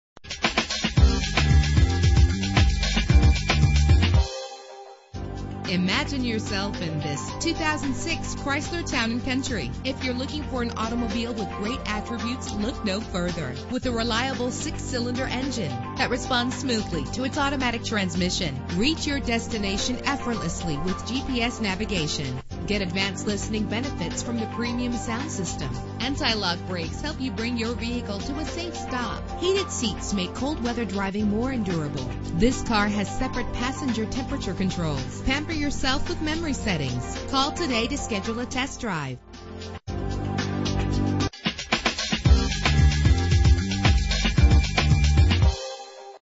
2006 Chrysler Town & Country - Henderson NV
Year: 2006
Make: Chrysler
Model: Town & Country
Trim: Limited
Engine: 3.8 liter 6 cylinder 12 valve
Transmission: 4-Speed Automatic
Color: Teal
Mileage: 66947
Address:
930 Auto Show Drive